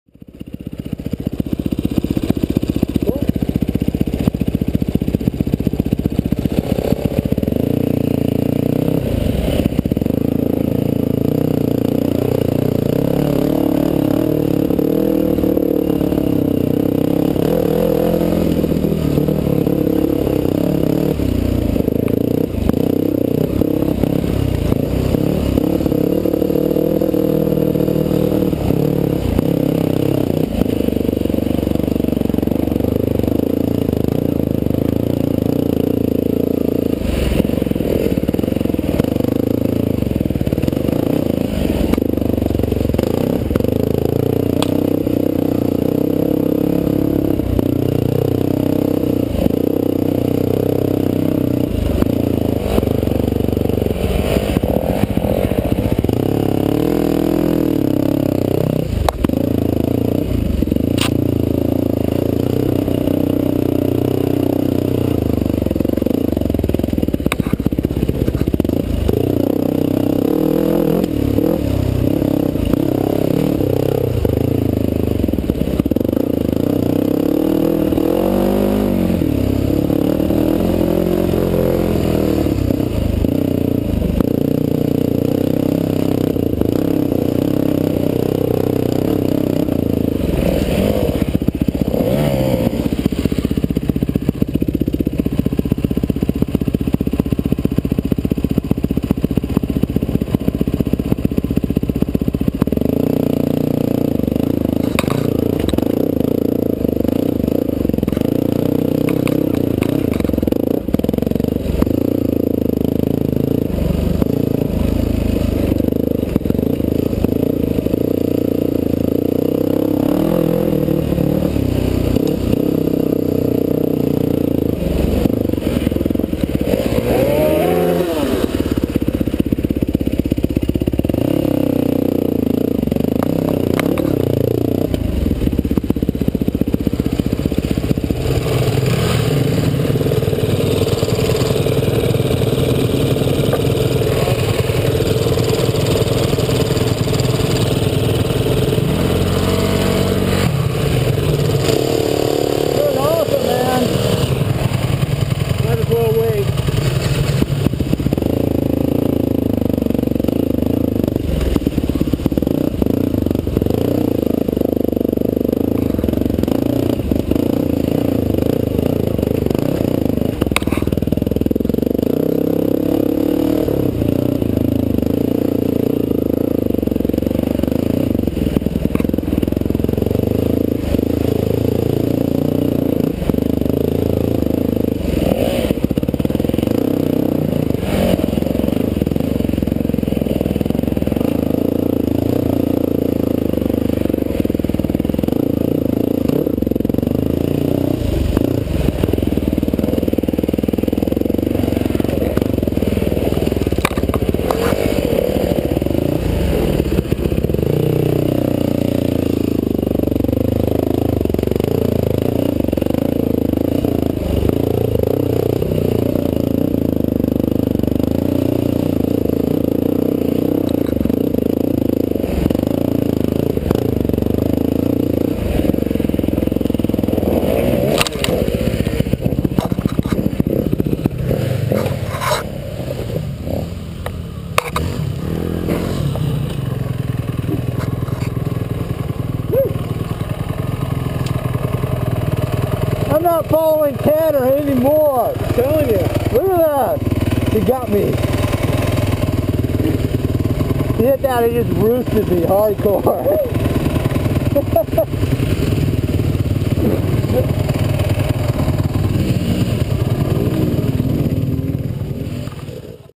Kilted Duck Mt Vernon
some gopro footage from the Kilted duck poker run on 4/22